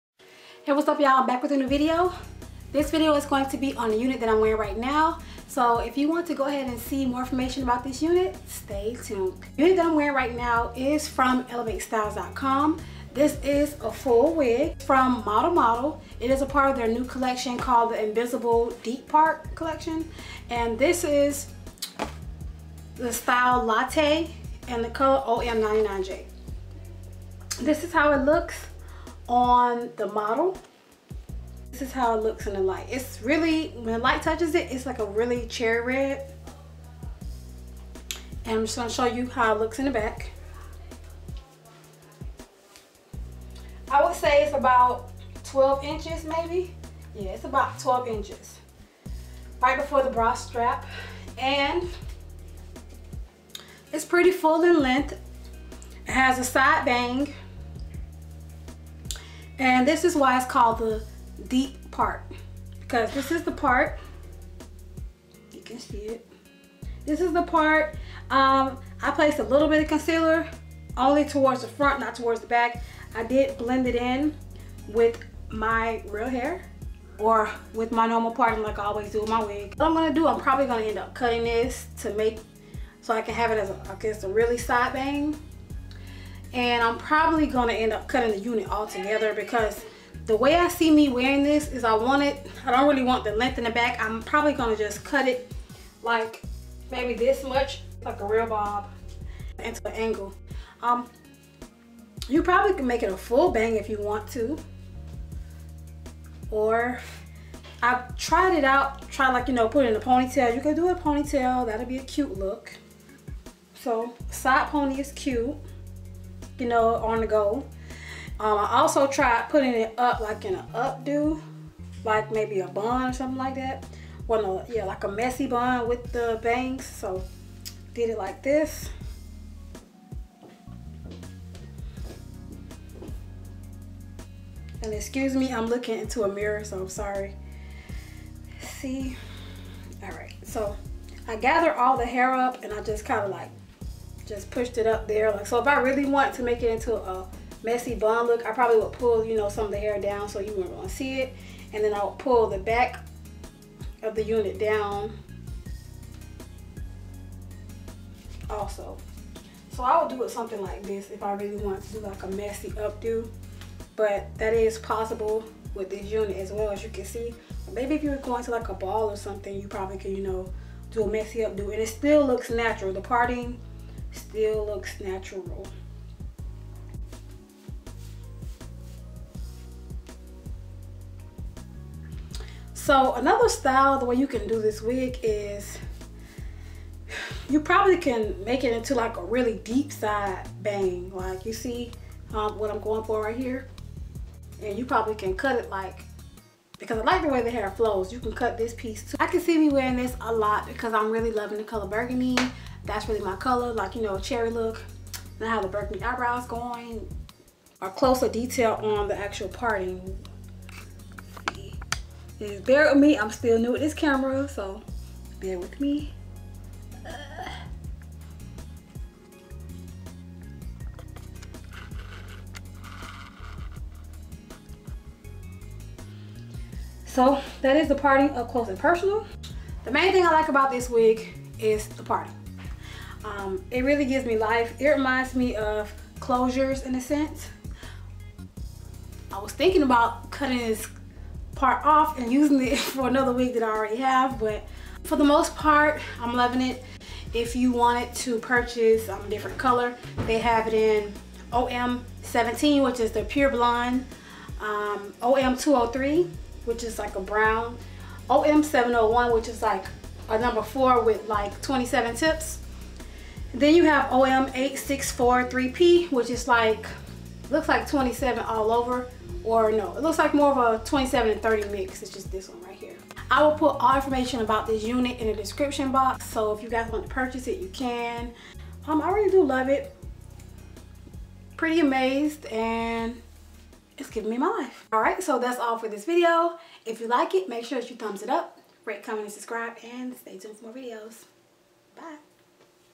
Model Model Deep Invisible Part Wig Latte | Elevatestyles.com
Forgot to mention that this wig has only elastic bands no combs. It also has ear tabs.
The unit can take heat up to 400 degrees. I used a low setting of 370 on the top to flatten it a bit on the top.
Its not shiny in person, however my lighting caused it to appear that way in this video.
The wig is currently sold out but will be available in 2 weeks.

(oh and ) Tell me how you like the quality of this video
Video was filmed with Canon Rebel T3.